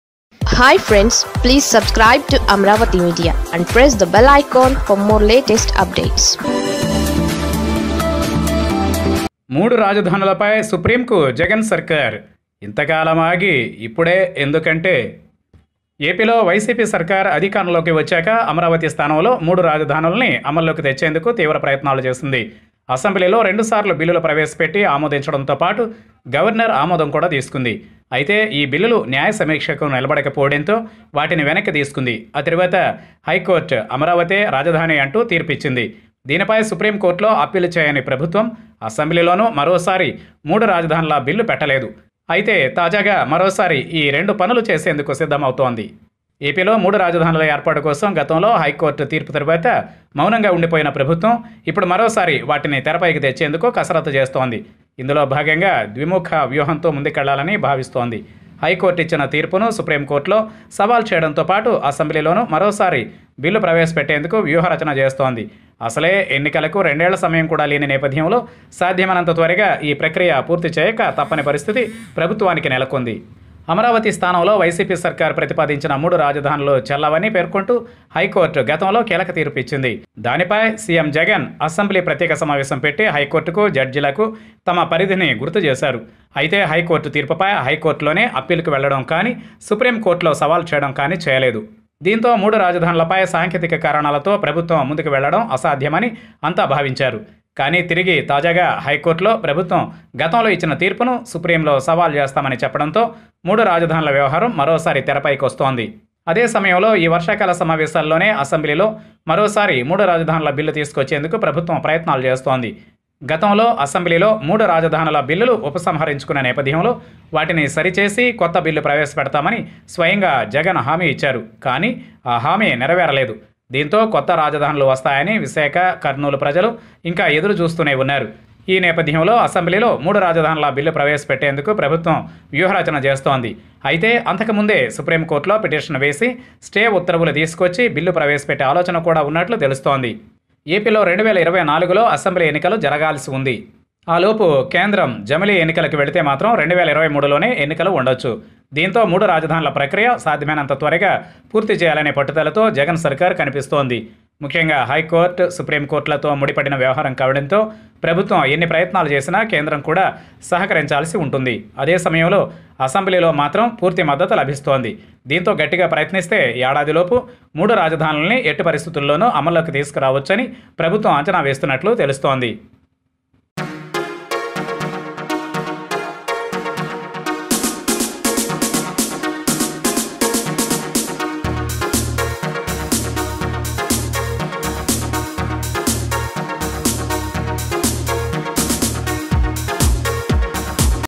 3 రాజధానులపై ద్విముఖ పోరుకు జగన్ సర్కారు రెడీ| Jagan Govt Plans To Move Supreme Court On 3 Capitals
3 రాజధానులపై ద్విముఖ పోరుకు జగన్ సర్కారు రెడీ| Jagan Govt Plans To Move Supreme Court On 3 Capitals